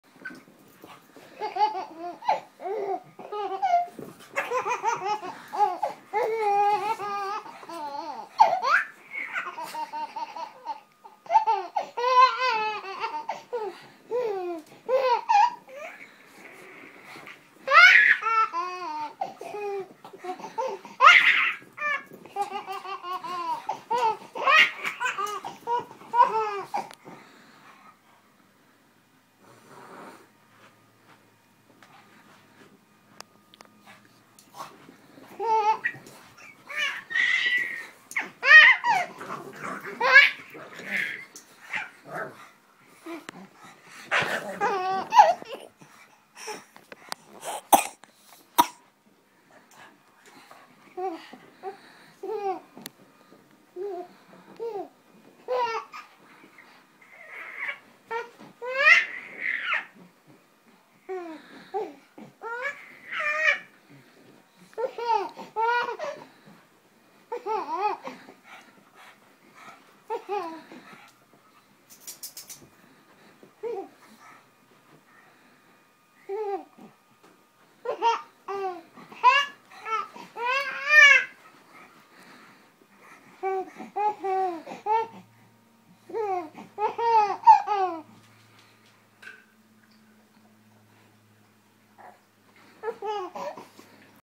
Baby thinks his pugs are HILLARIOUS!
Kian laughing at his pugs chowder and Bug